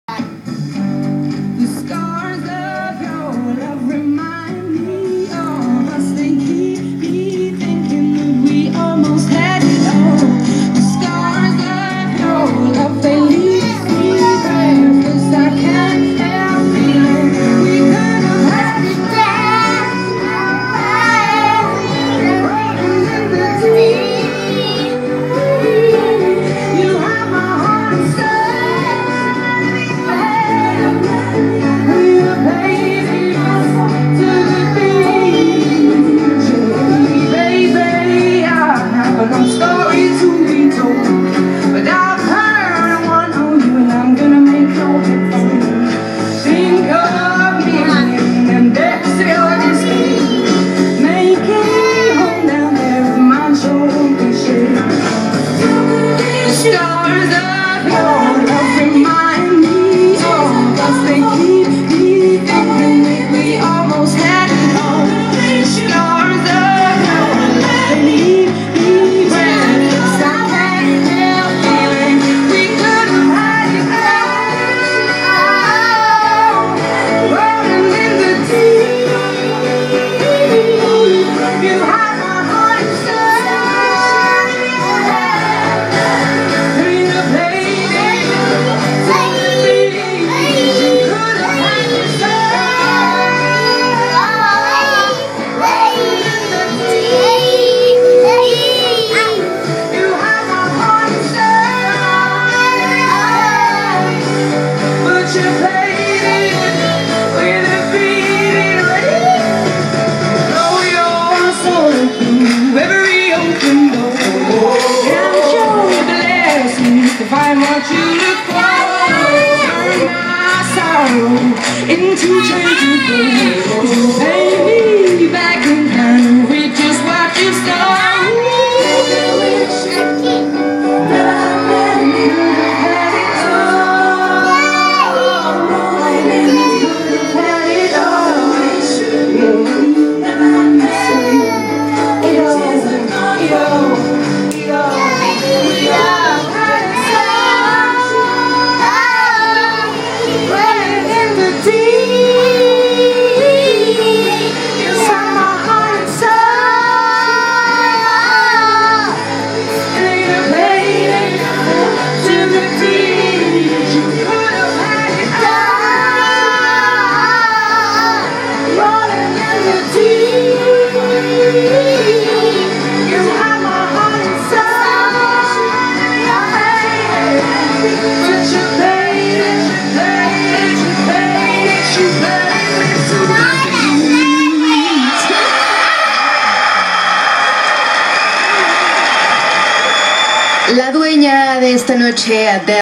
Val y so cantan
Mis hijas cantando con adele en los Grammy